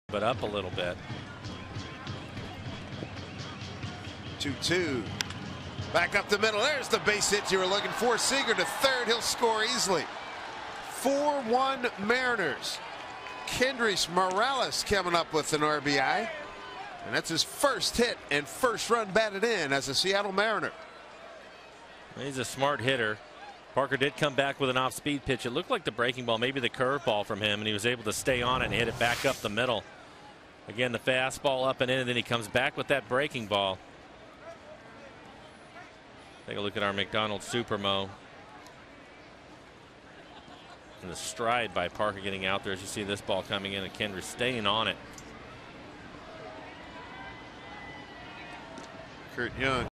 Morales' RBI single
4/2/13: Kendrys Morales drives a single up the middle in the fifth to record his first RBI as a Mariner and extend the lead to 4-1